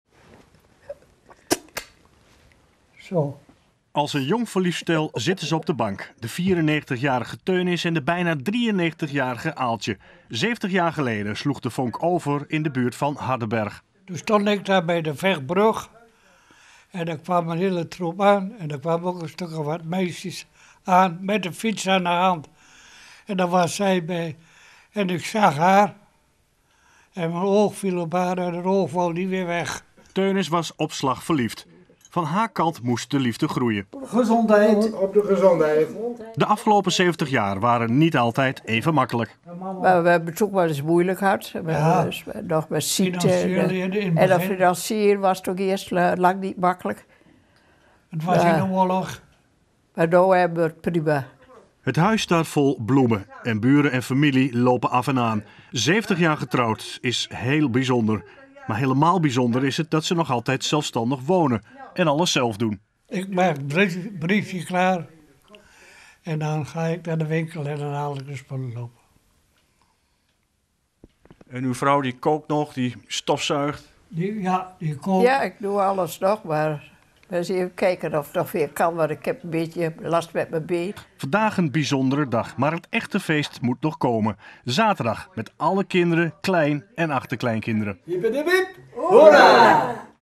Echtpaar uit Almelo viert 70-jarig huwelijk
Het komt niet zo vaak voor en dat maakt het des te meer bijzonder: een 70-jarig huwelijk.

Teunis en Aaltje Michel uit Almelo vieren vandaag hun platina huwelijk. Een geheim voor hun huwelijk hebben de 94-jarige Teunis en de bijna 93-jarige Aaltje niet: ze noemen het ware liefde.